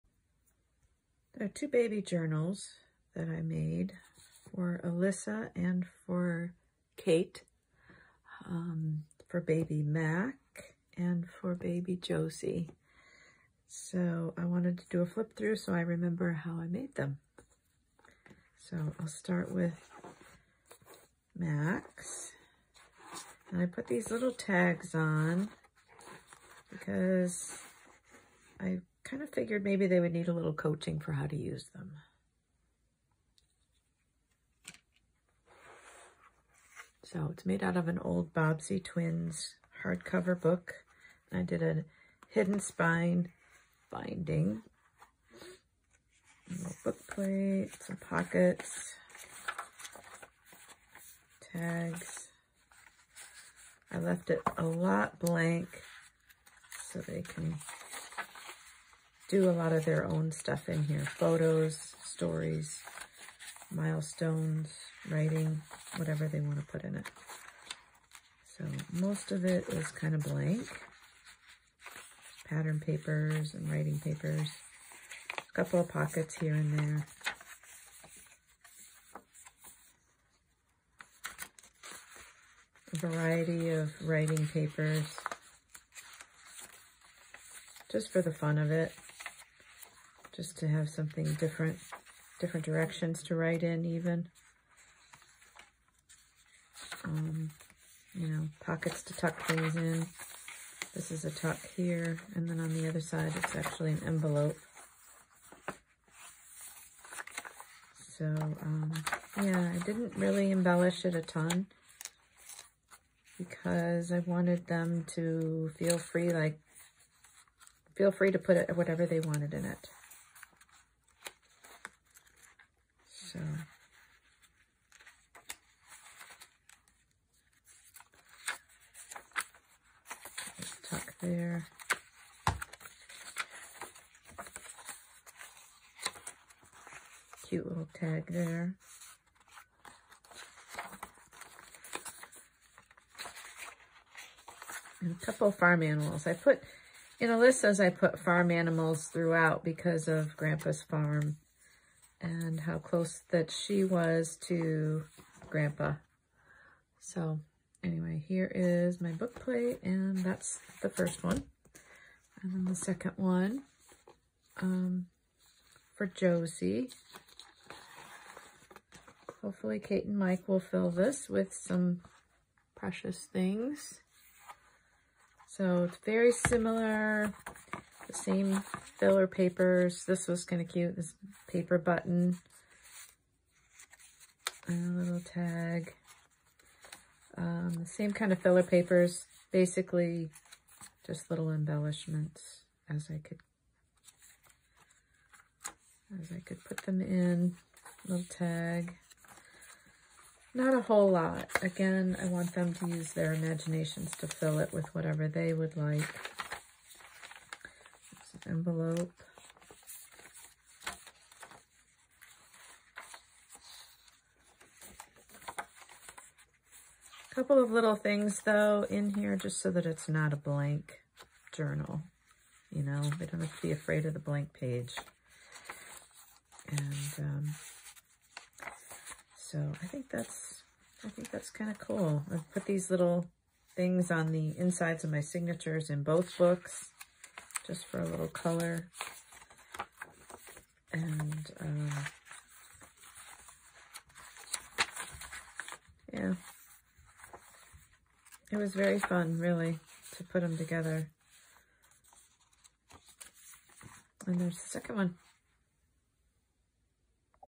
Tiny Moments Journals
A flip-through of a couple of Baby Books I made in a junk journal style. Made with vintage book covers and a variety of writing papers and colorful scrapbooking papers, tags, tucks, pockets, and envelopes throughout.